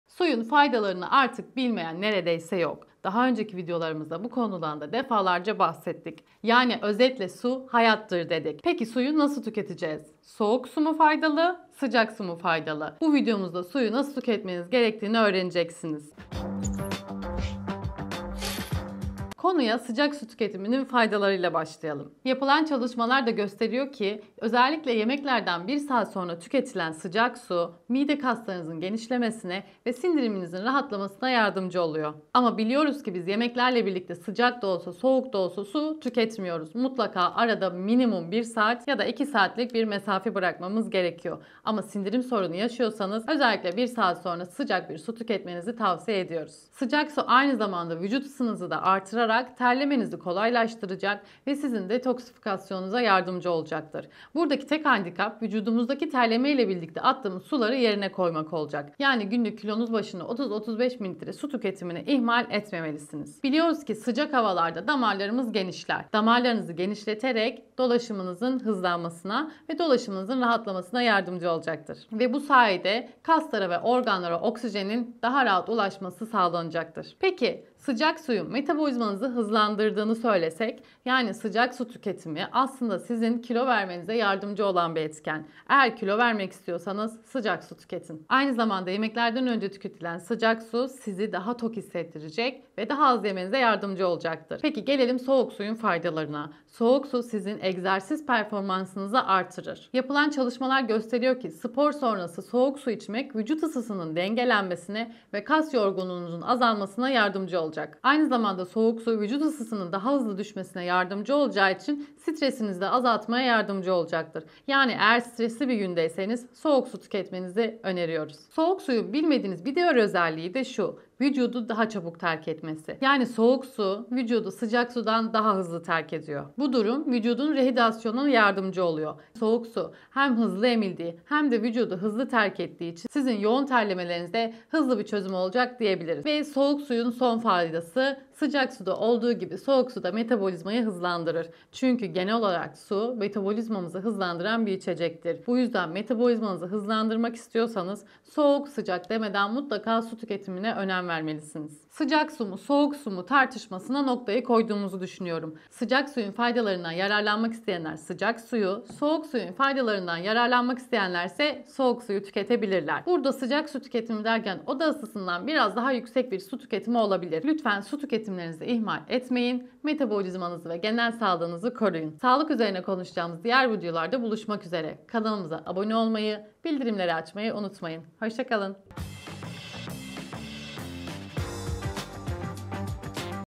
Soğuk Su Mu İçilmeli? Sıcak Su Mu?
Su, yaşamın olmazsa olmaz kaynağıdır ve bedenimizin temel işlevlerini yerine getirebilmesi için sürekli olarak suya ihtiyaç duyarız. Ancak, suyun hangi sıcaklıkta içilmesi gerektiği konusunda farklı görüşler bulunuyor. Bu makalede, suyun sıcak ya da soğuk içilmesinin vücut üzerindeki etkilerini ele alacağız.

Bu videoda su tüketimi ile ilgili merak ettiğiniz "Sıcak su faydalı mı? Yemeklerle su içilmeli mi? Sıcak su tüketimi dolaşımı hızlandırır mı? Sıcak su tüketimi kilo kaybı sağlar mı? Spor sonrası soğuk su içmek faydalı mı? Soğuk su strese iyi gelir mi? Soğuk su rehidyasyon sağlar mı? Soğuk su metabolizmayı hızlandırır mı?"sorulara yanıt verdik.

00:20 Sıcak su faydalı mı?
00:40 Yemeklerle su içilmeli mi?
01:10 Sıcak su tüketimi dolaşımı hızlandırır mı?
01:30 Sıcak su tüketimi kilo kaybı sağlar mı?
01:40 Spor sonrası soğuk su içmek faydalı mı?
01:50 Soğuk su strese iyi gelir mi?
02:10 Soğuk su rehidyasyon sağlar mı?
02:20 Soğuk su metabolizmayı hızlandırır mı?

Sıcak ve soğuk su tüketimi ile ilgili merak ettiğiniz soruları makalemde bulabilirsiniz⤵️

Diğer videolarımıza göz atmayı unutmayın.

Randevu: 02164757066
Adres: Doç.Dr.Muhammed Keskin, Kadıköy, İstanbul